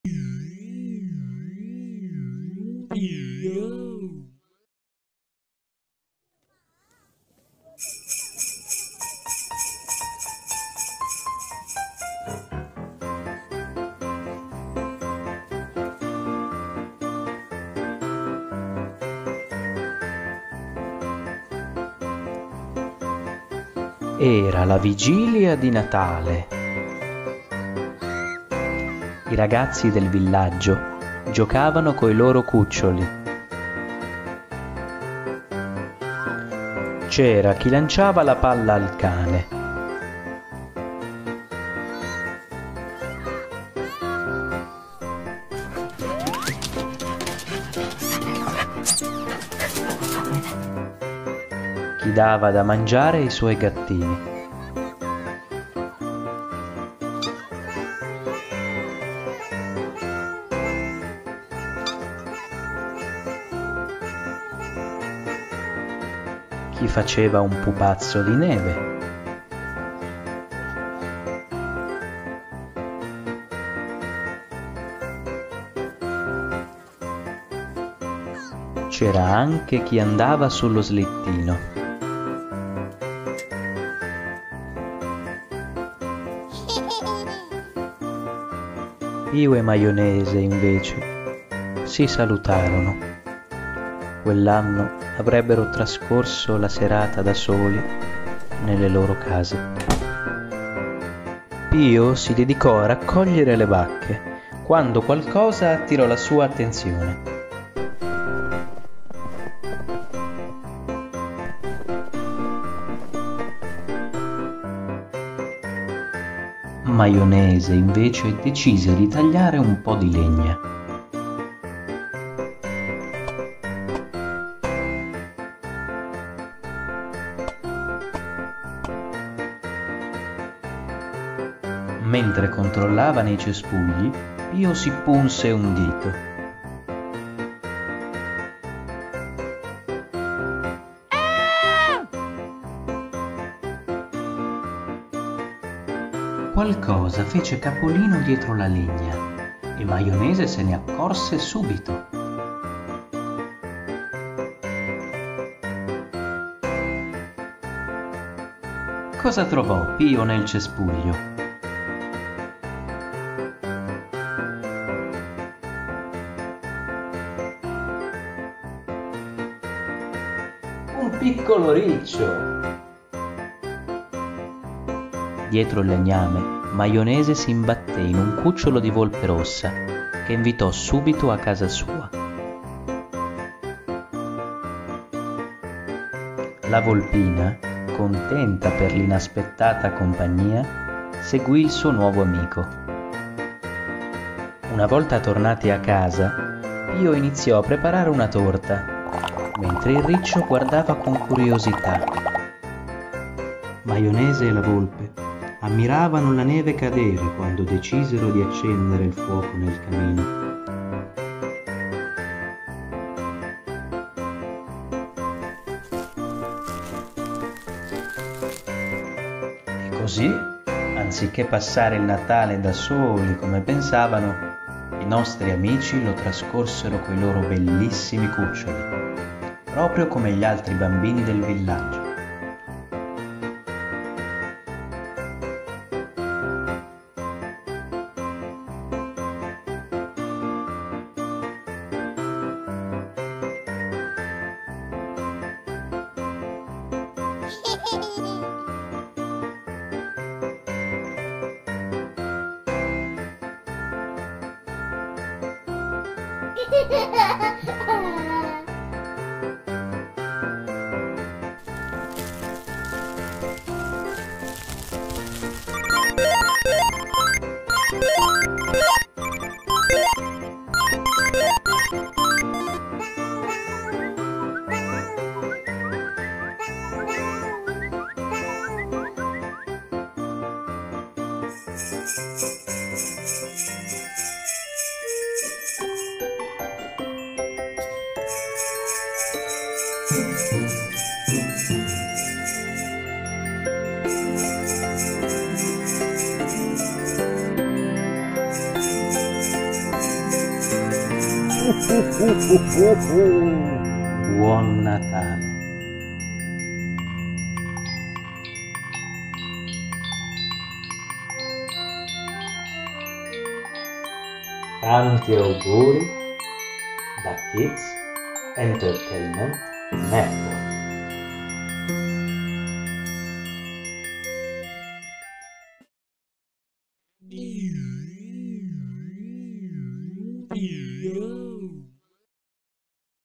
La storia di Natale di Pio e Maionese
Cartone animato che racconta della vigilia di Natale...episodi casuali creano una bellissima amicizia tra uomo e animali.